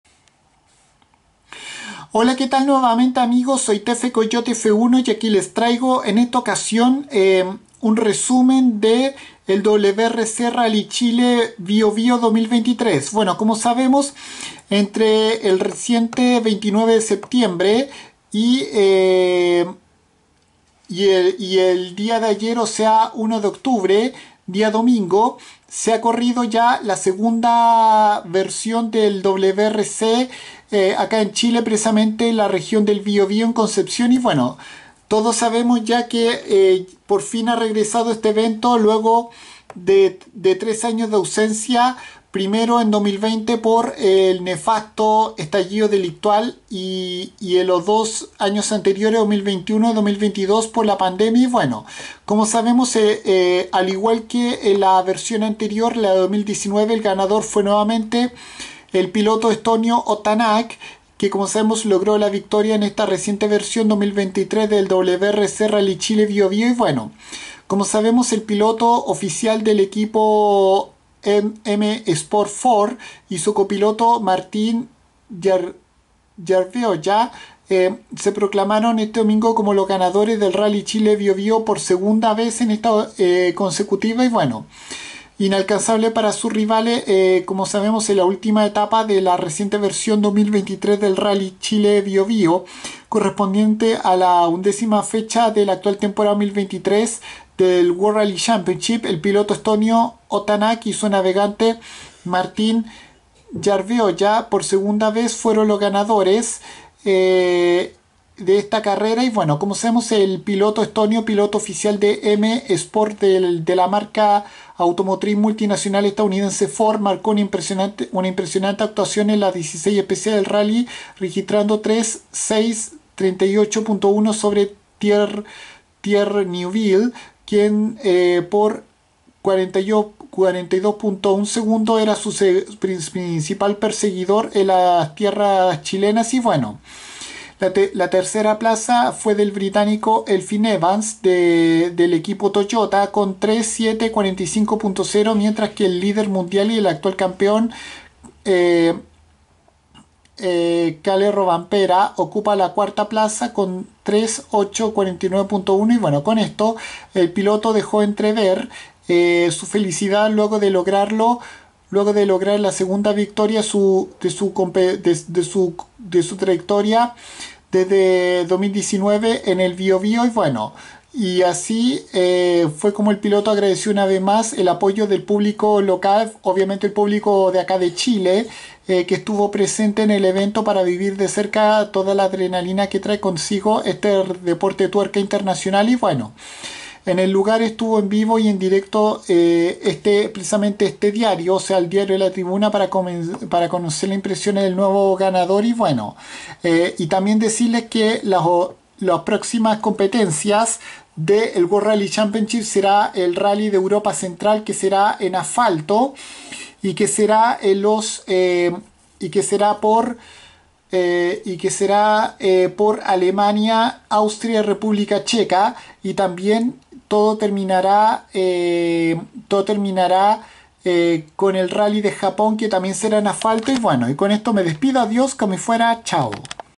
Resumen WRC Rally Chile Bio Bio 2023
La competencia del World Rally Championship disputada en el Bio Bio acá en Chile fue del estonio piloto oficial de M Sport Ford Ott Tanak por 2da vez consecutiva

Además las próximas competencias del WRC serán el Rally de Europa Central(Alemania Austria y República Checa) y el Rally de Japón las últimas fechas de la actual Temporada 2023 y ambas serán sobre asfalto